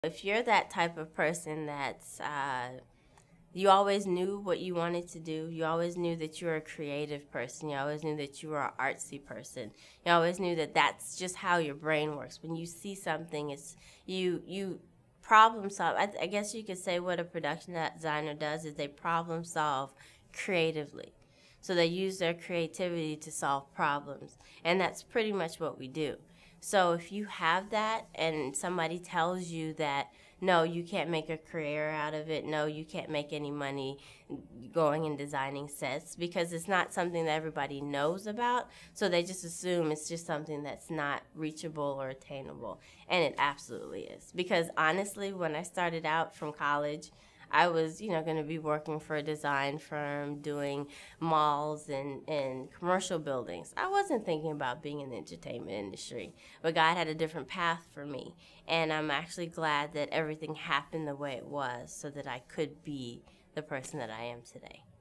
Production Designer: Considering Production Design as a Career - Shauna Williams Career Girls
Watch Shauna Williams, Production Designer, share valuable career guidance and life advice with girls Learn how to become a Film/TV Production Designer and watch her full interview